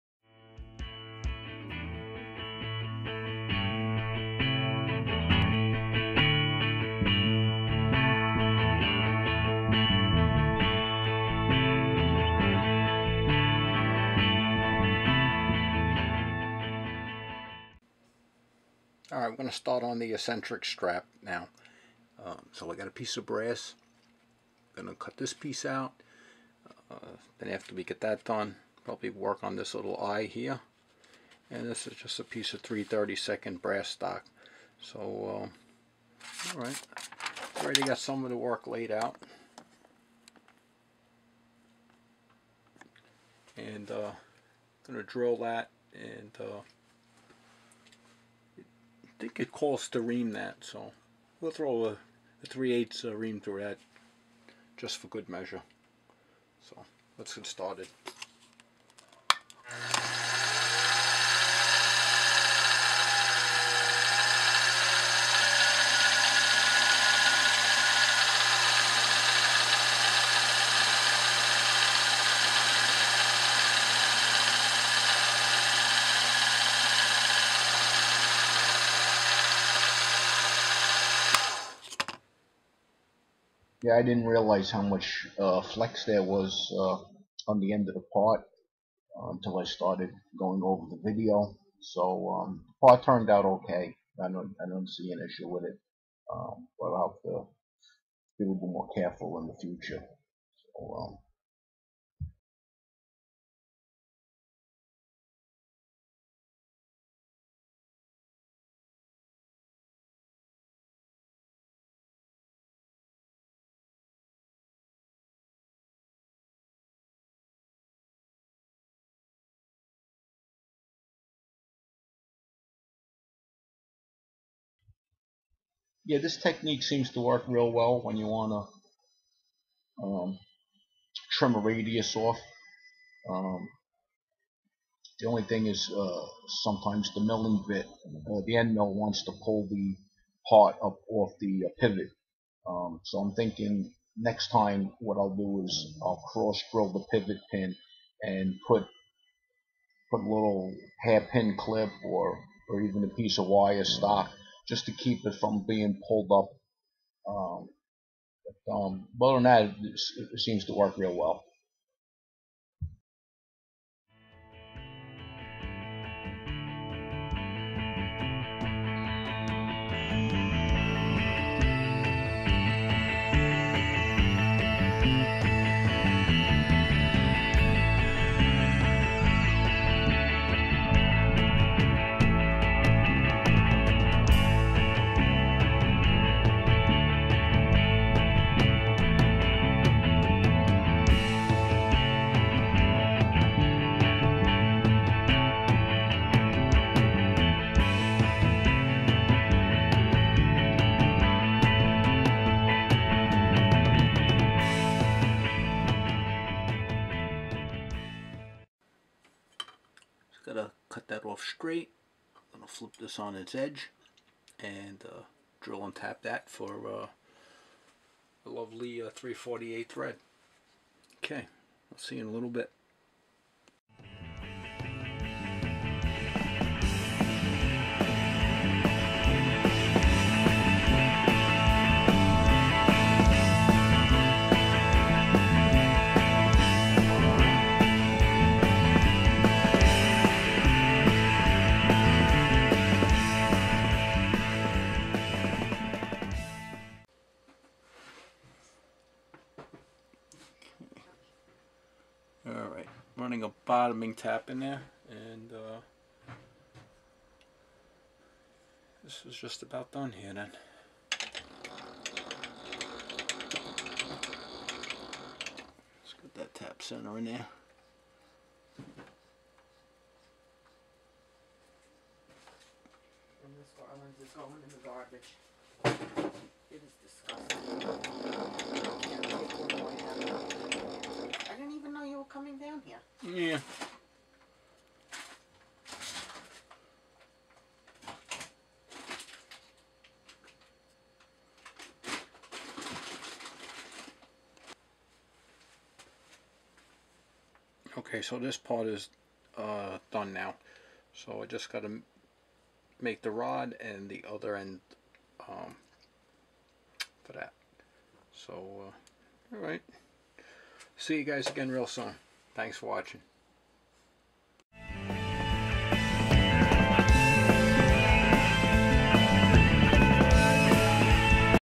Elmer Verberg Beam Engine Eccentric Strap
Machining the eccentric strap for the Elmer Verburg #24 Beam Engine